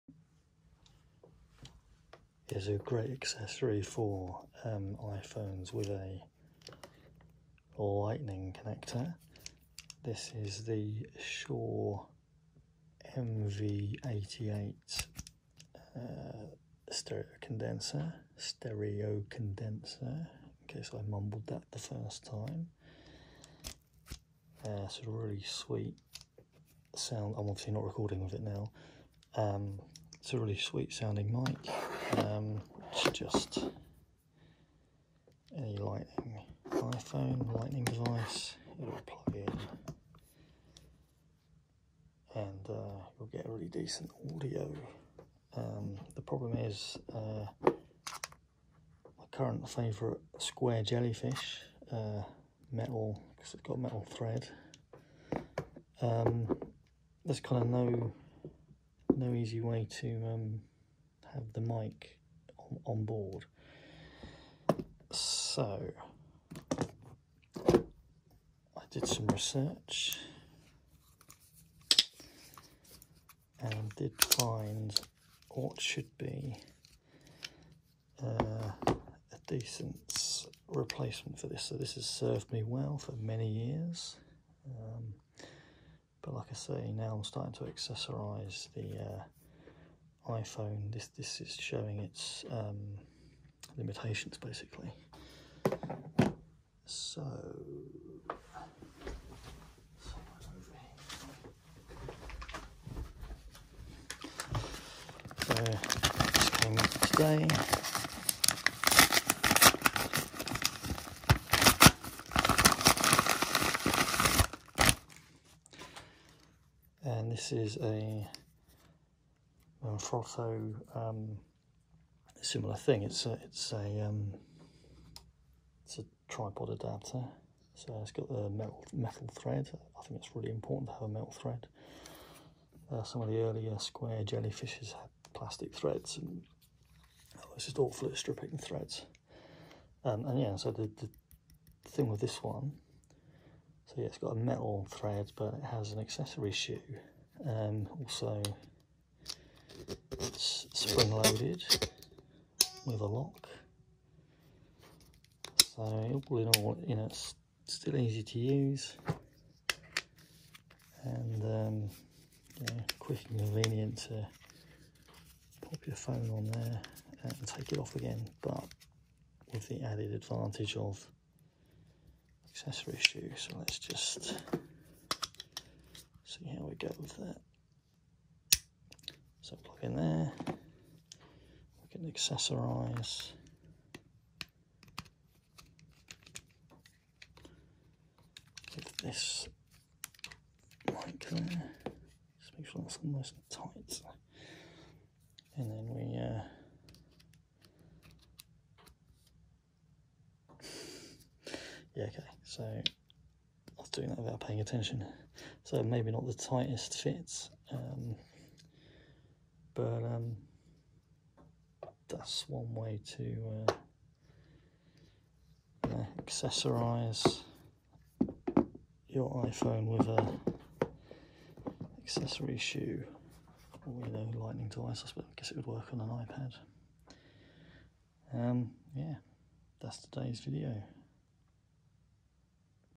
microphone accessory shoe tripod adapter for smartphones / Manfrotto Universal Smartphone Clamp
if you have a microphone you want to mount on your smartphone and tripod adapter but current gear isn't cutting it for you, here's something to consider from manfrotto

Manfrotto Universal Smartphone Clamp, Pro Version (MCPIXI)
Holds smartphone with widths of 2.36" To 4.09"
Universal tripod and lighting connections
Double lock mechanism: spring lock and side lock
Two ¼ attachments: for landscape and portrait mode
Ergonomic Italian design, Compact and lightweight